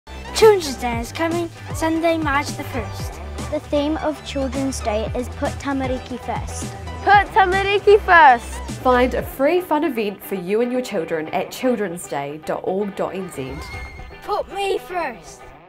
Children's Day is Sunday 1 March
Children's Day is Sunday 1 March. Celebrate with the children in your life by coming along to an event. Find out what events are on in your region